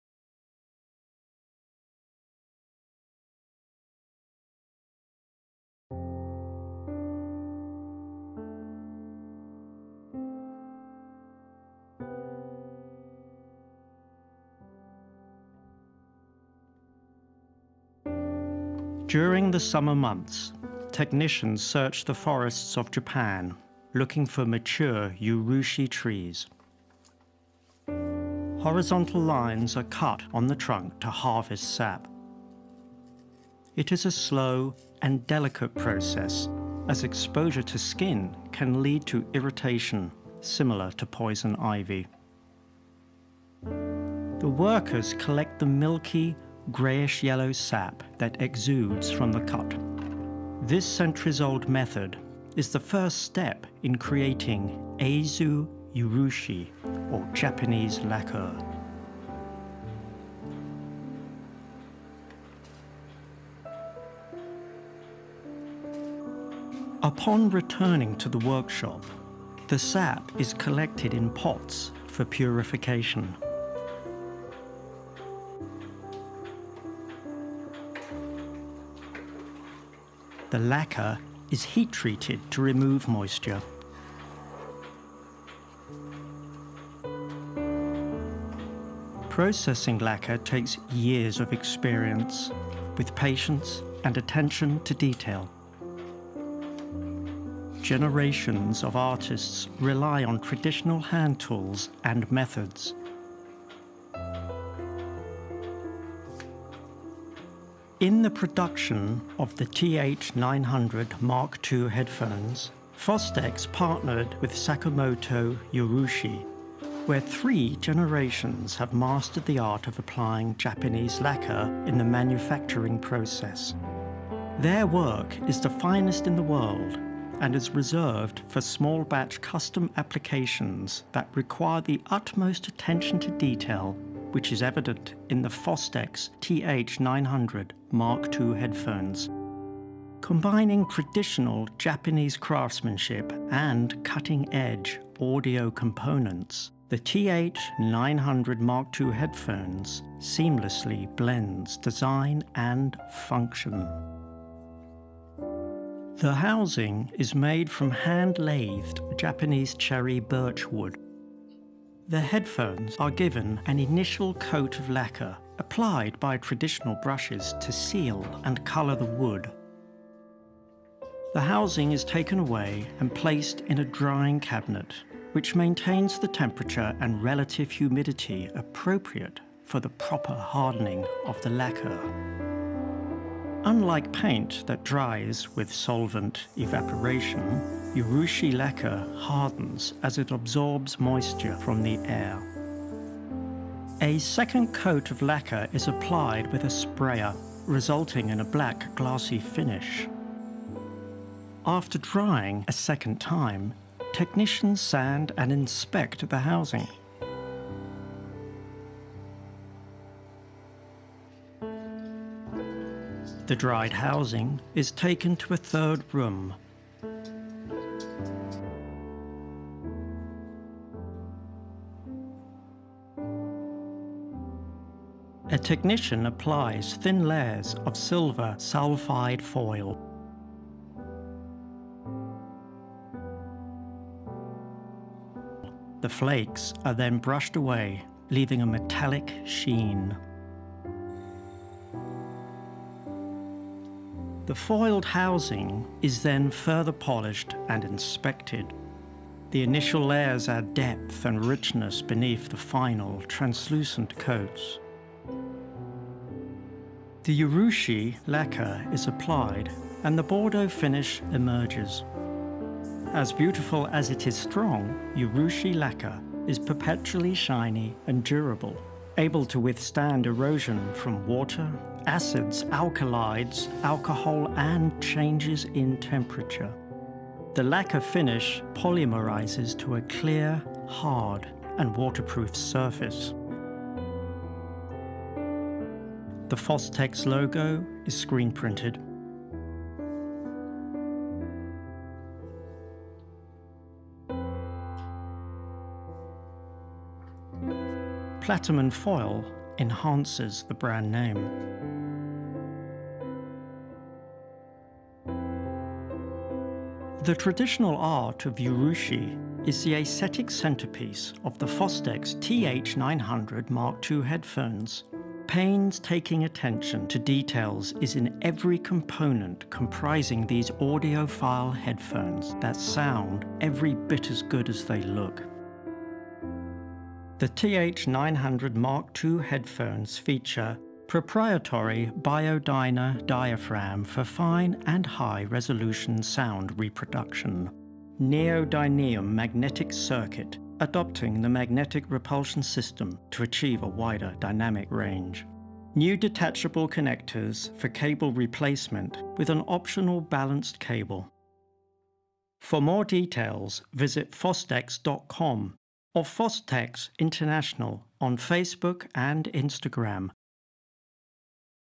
Making of the Fostex TH900
This video shows the masterful process of creating the ear cups and the Japanese traditional "Urushi" lacquer by a 110 years old artisan group in Bordeaux. This housing is used for the Fostex TH900 (shown in video) and Fostex TH900MK2 Premium Stereo Headphones.

TH900mk2 is the second generation of the TH900, one of the most renowned premium dynamic headphones in the market, which newly features detachable connectors for replace-ment of the standard unbalanced cable (equivalent to model ET-H3.0N7UB).  The optional balanced cable, model ET-H3.0N7BL will also be sold separately to meet growing demands for balanced connection with various headphone amps offering balanced 4-pin XLR out.